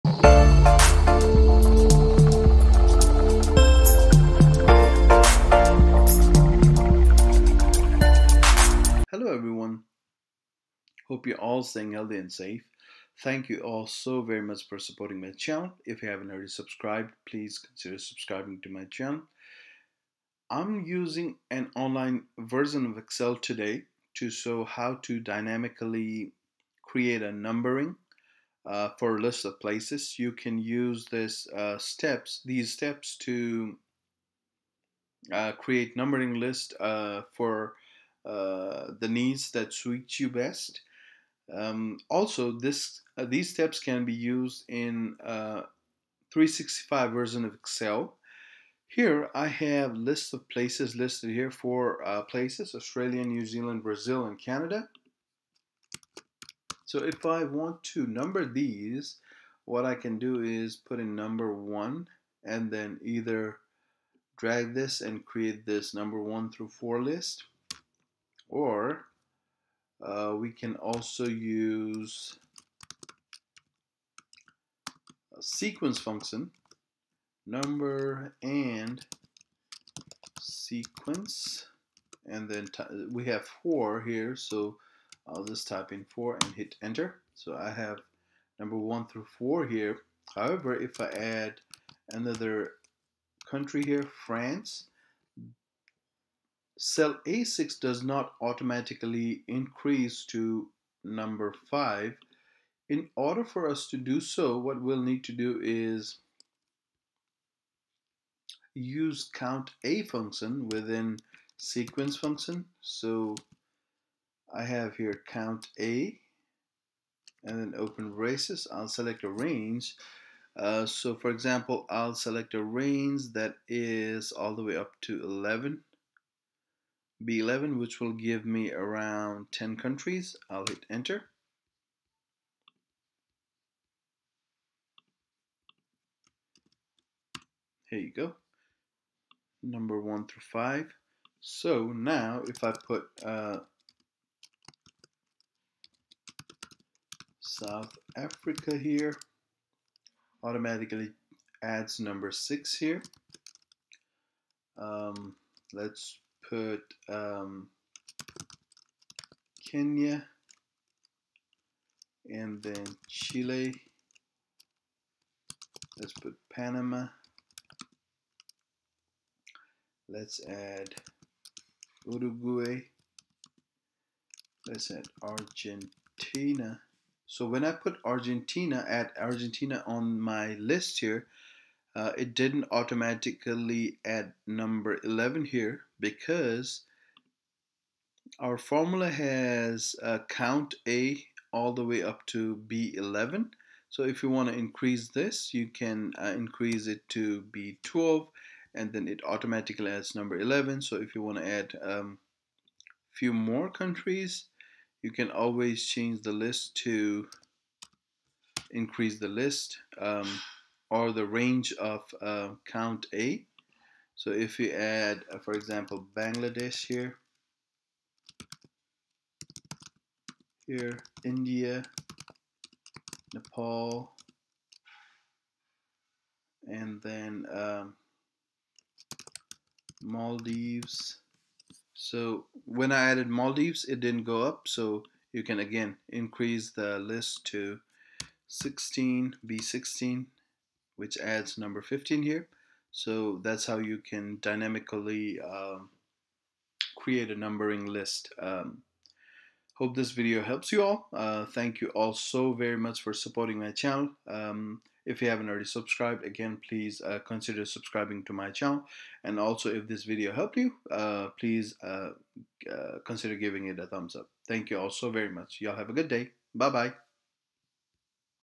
How to create dynamic numbering list in Excel using sequence and counta
Step by step tutorial on how to create dynamic numbering list in Excel using sequence and count a function. Hope this helps you all!